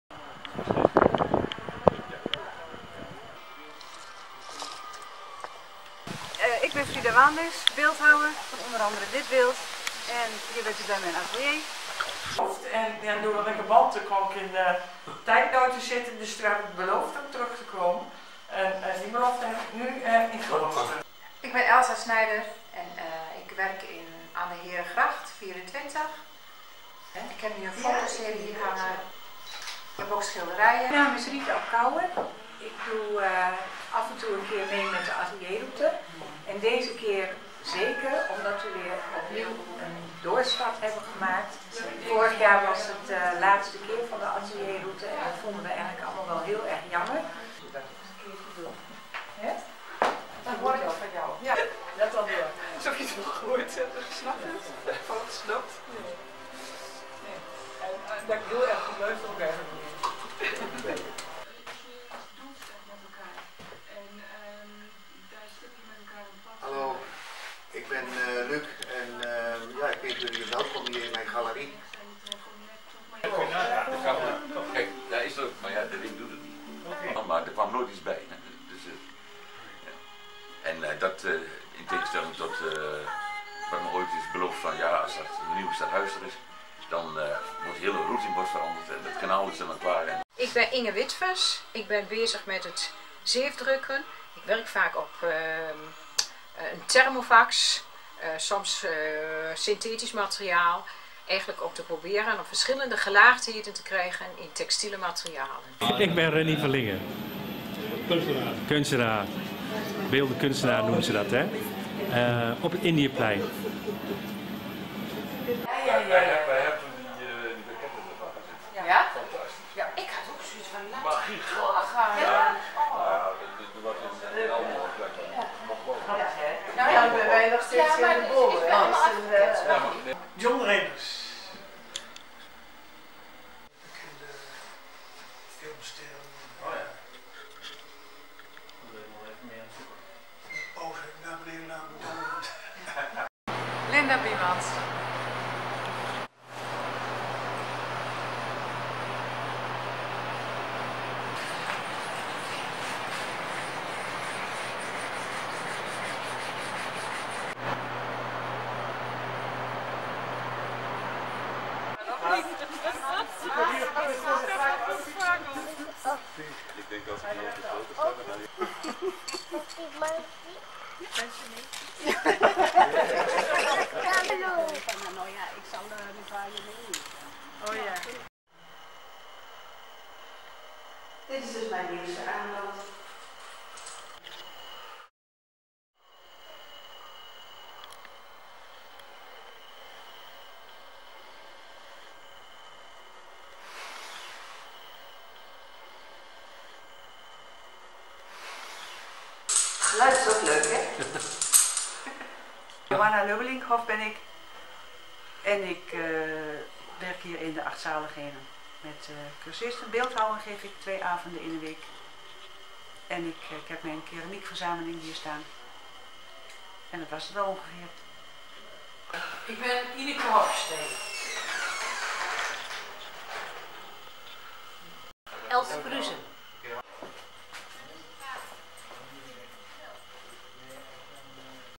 Kijkje nemen bij kunstenaars tijdens Open Atelier Route
Afgelopen weekend stond Almelo in het teken van de Open Atelier Route.
Na de vorige editie was er onduidelijkheid over het voortbestaan. De stichting Open Ateliers Almelo sloot zich aan bij het landelijke atelierweekend van Stichting Kunstweek. Een doorstart van het jaarlijkse evenement was zodoende een feit.